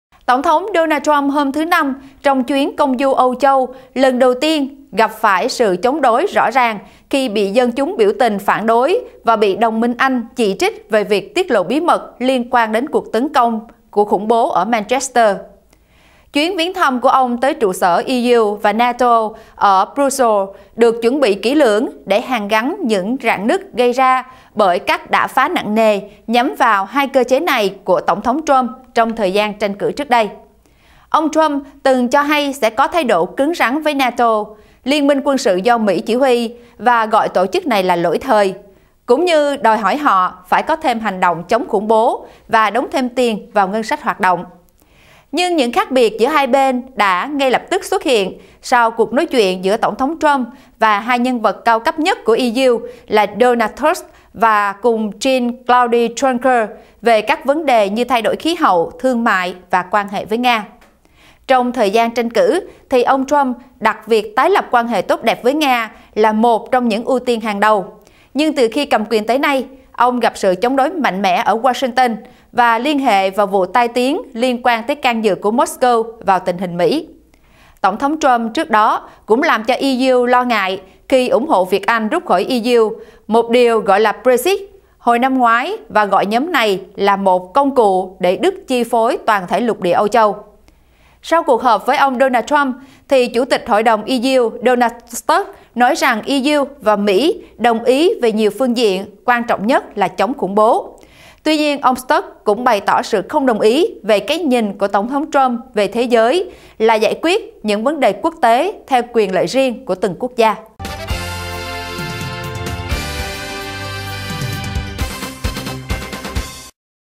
Tổng Thống Donald Trump đến Brussels, tạo cảm thông với EU và NATO
Tổng Thống Donald Trump hôm Thứ Năm, trong chuyến công du Âu Châu lần đầu tiên, gặp phải sự chống đối rõ ràng khi bị dân chúng biểu tình phản đối và bị đồng minh Anh chỉ trích về việc tiết lộ bí mật liên quan đến cuộc tấn công của khủng bố ở Manchester.